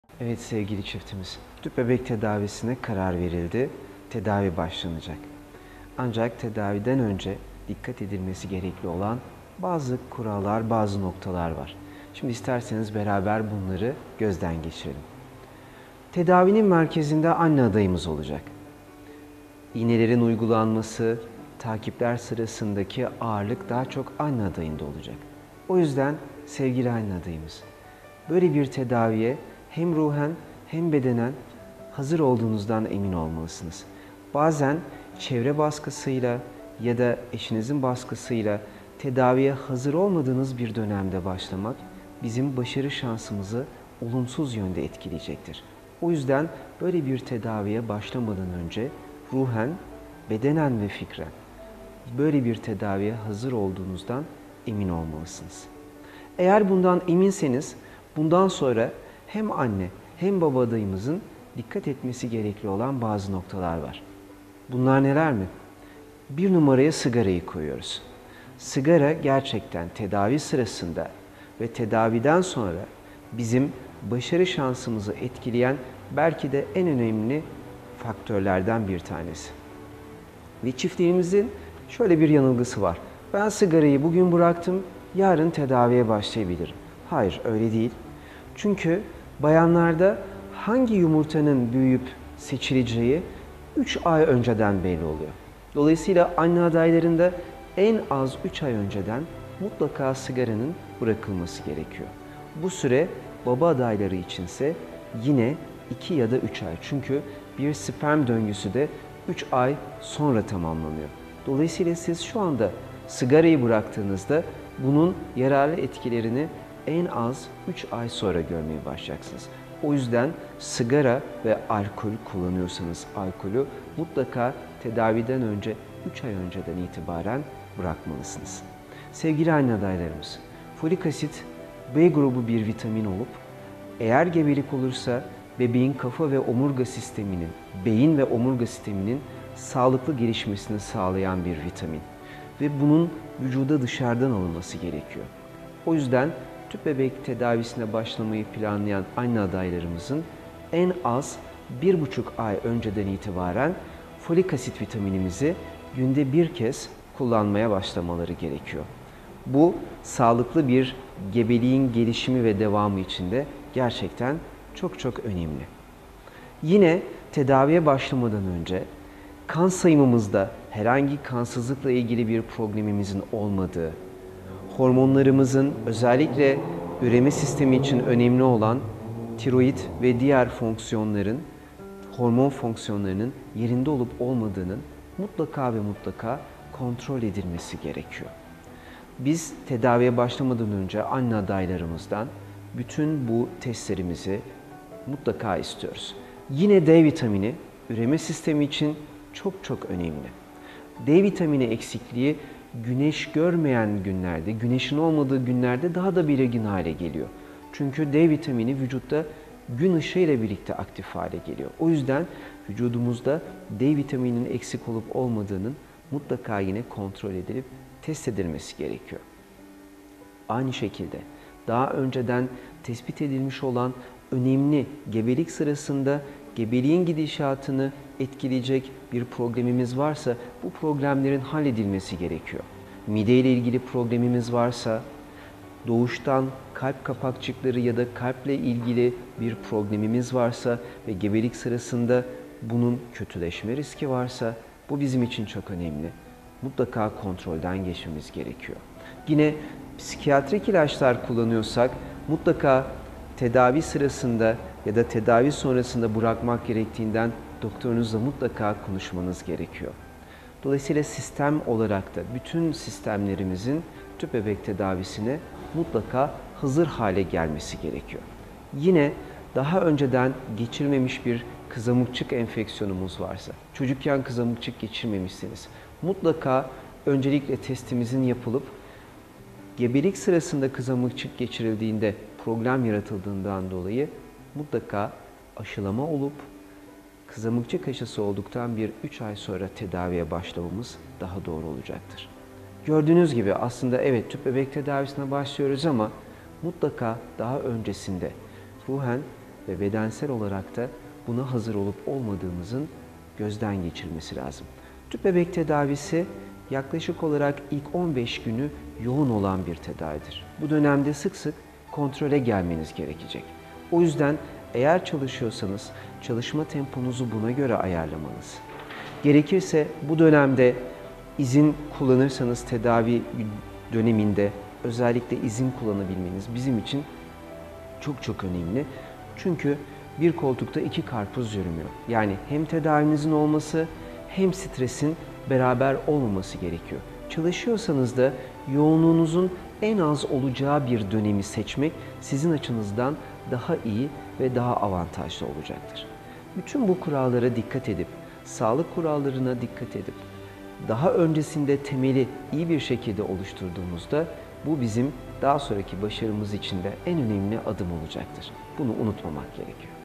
Tüp Bebek Tedavisine Hazırlanırken Nelere Dikkat Edilmeli - Op. Dr. Emre Karatekelioğlu
Teşvikiye Mahallesi, Hakkı Yeten Caddesi, Vital Plaza Fulya
No: 23 Kat: 5 Daire: 516
Fulya - Şişli / İSTANBUL